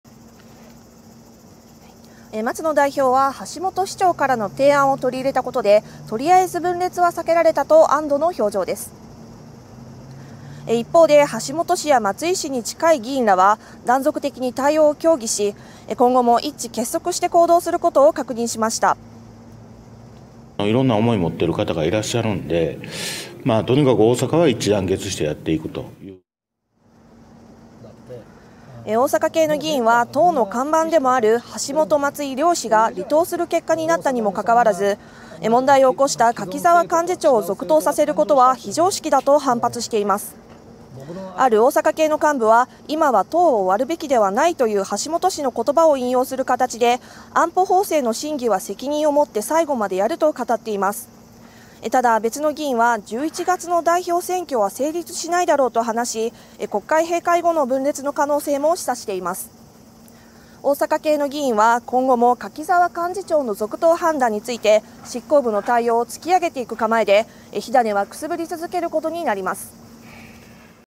火種くすぶる“今後の維新”2人の離党でどうなる？(15/08/27)
いったん収束した形での維新の党の内紛ですが、今後はどうなるのでしょうか。　（政治部・岡香織記者報告）　松野代表は橋下大阪市長からの提案を取り入れたことで、とりあえず分裂は避けられたと安堵（あんど）の表情です。